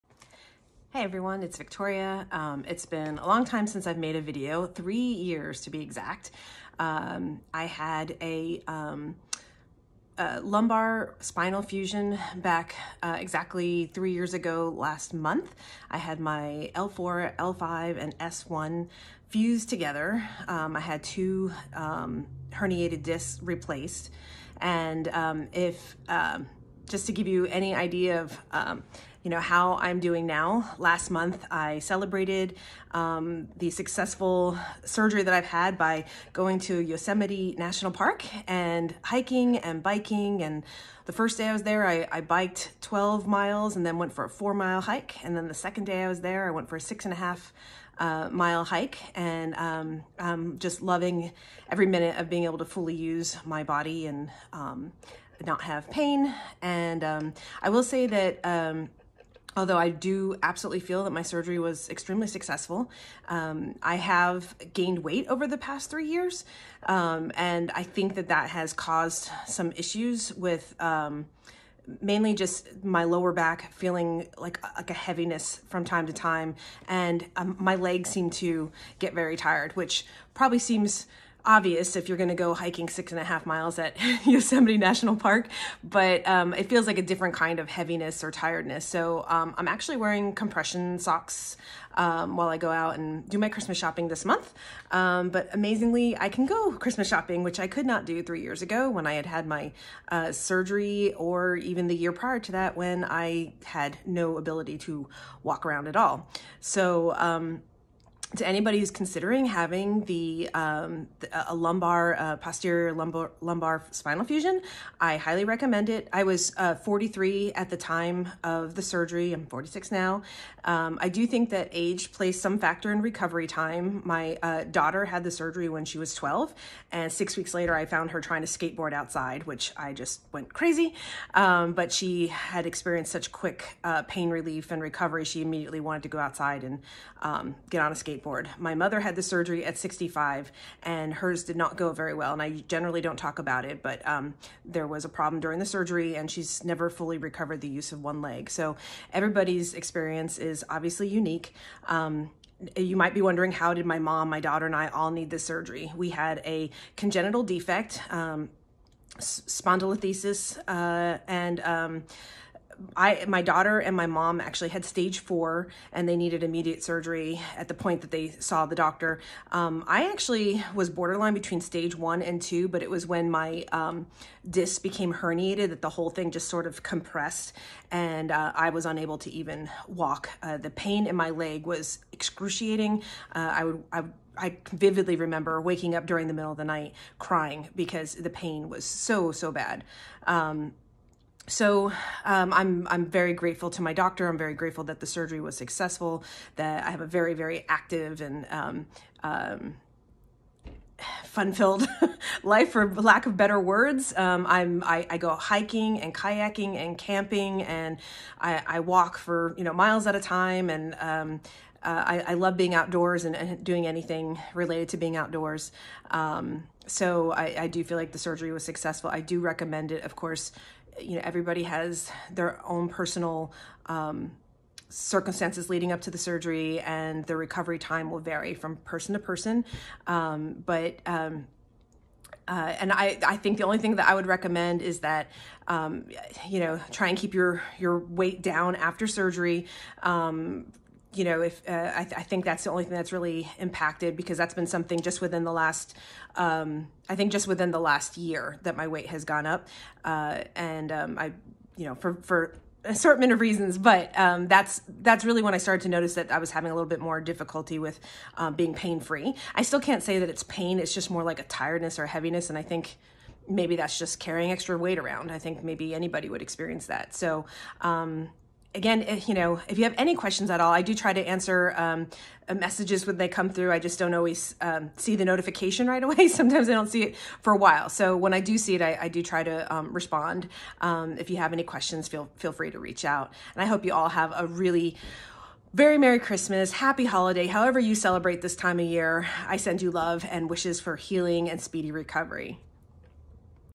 UPDATE: 3yr post-recovery Posterior Lumbar Spinal Fusion Surgery
I’ve been asked by several people how I’m doing today, three years after having a posterior lumbar spinal fusion. I consider my surgery a wonderful success and I want to share the message that full recovery and returned quality of life is possible!